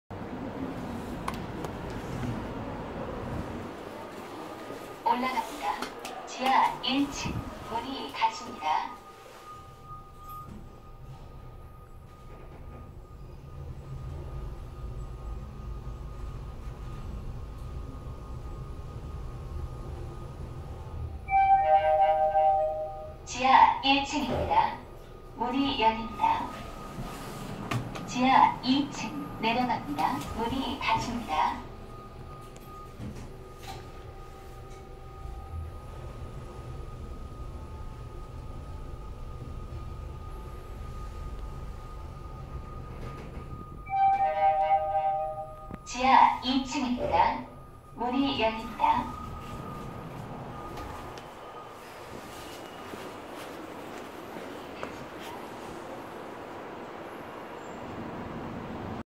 서울특별시 마포구 양화로 지하188 (동교동) 한국철도공사서울본부경의선홍대입구역 지평방향 현대엘리베이터 탑사기 (화질개선 재촬영)
서울 출사가 끝나고 집에가기전에 탑사했습니다.

* 여기는 승강장이 혼잡할 수 있으니 탑사시 주의하시기 바라며, 이 엘리베이터 이용하시면 2호선과 공항철도와 연결되지 않습니다.